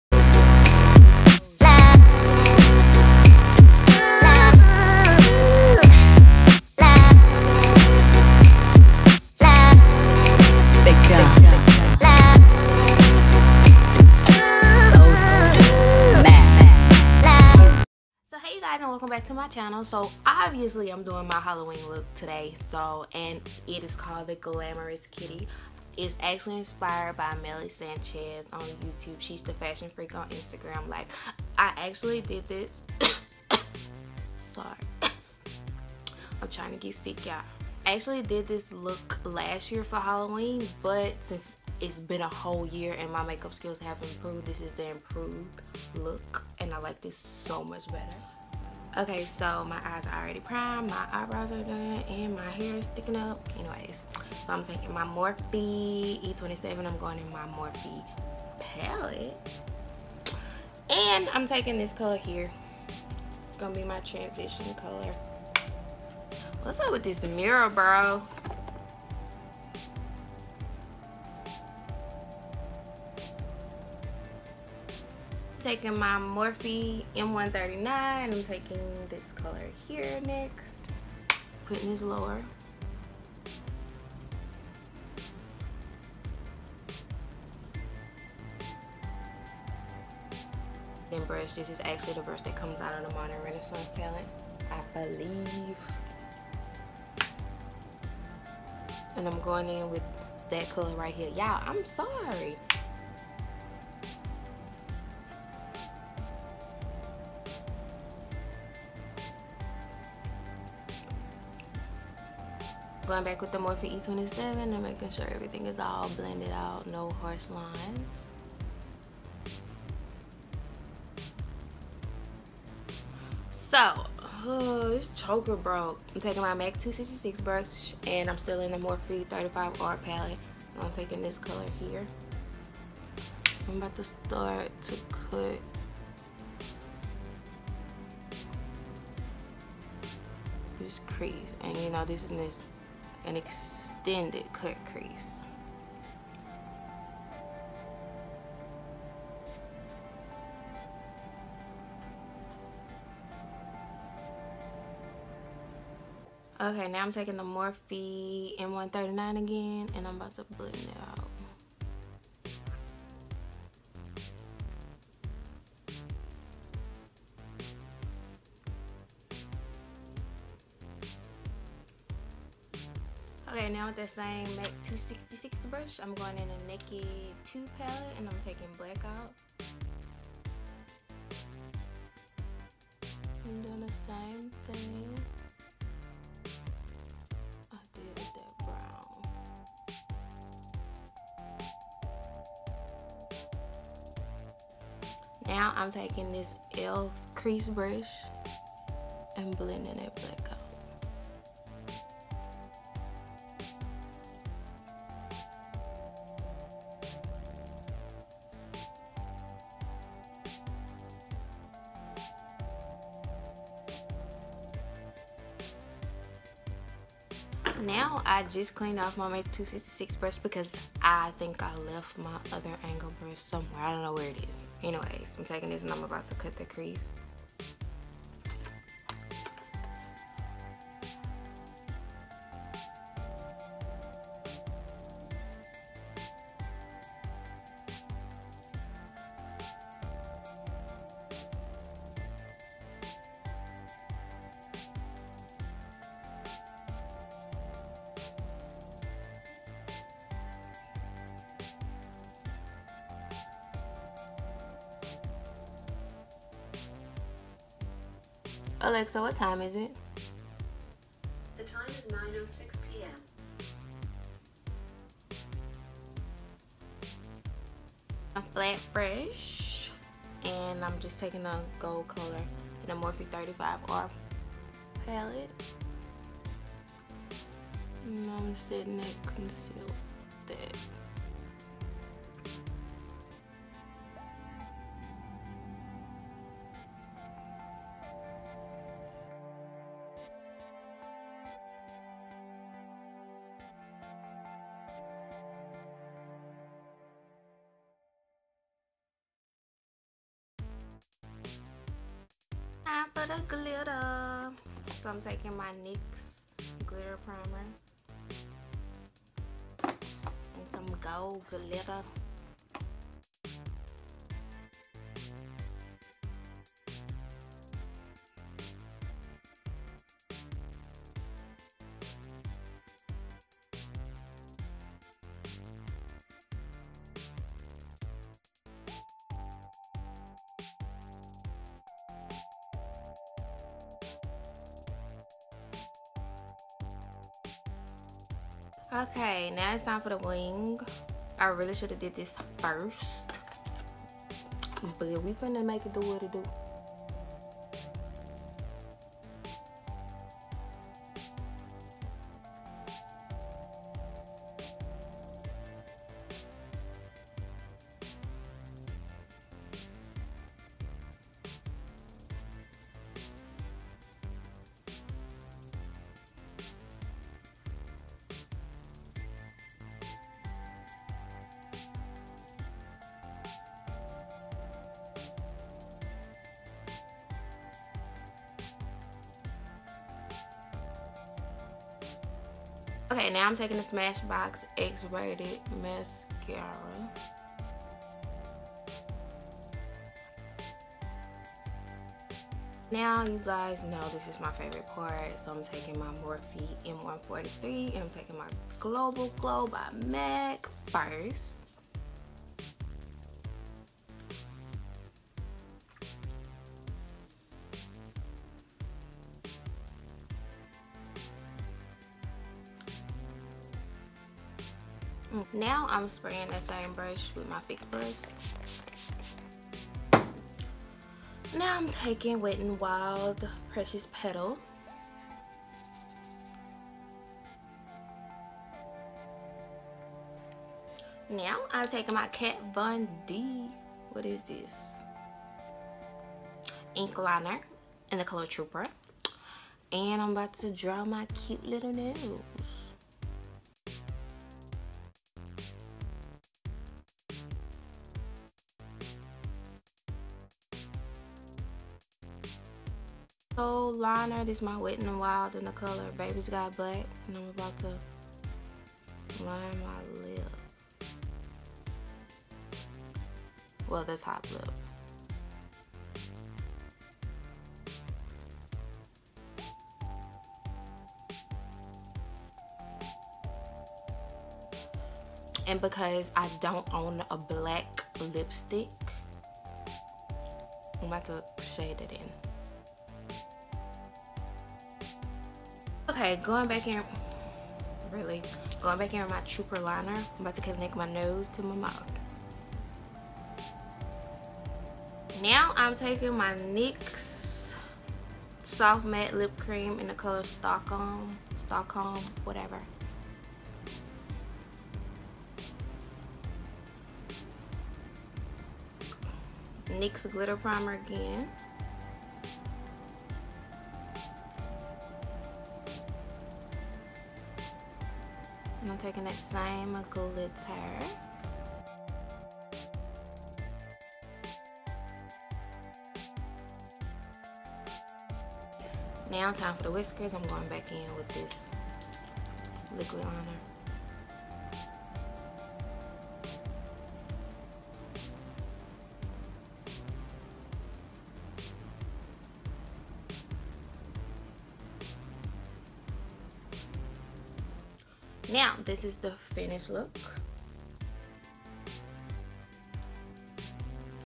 Glamorous Kitty ♡ Makeup Tutorial | Melanie Kontour
Face Products ❤︎

Maybelline Fit Me Foundation: Spicy Brown ❤︎
LA Girl HD Pro Coverage Foundation-Warm Caramel ❤︎
Coty Airspun Loose Powder- Translucent❤︎
Cover Girl Queen Collection Bronzer -Ebony Bronze❤︎
Wet n Wild Precious Petals Highlighter❤︎
Fix+❤︎

Eye Products❤︎
Juvia Place-Masquerade Palette♥︎
Smashbox X-Rated Mascara
Gold Gitter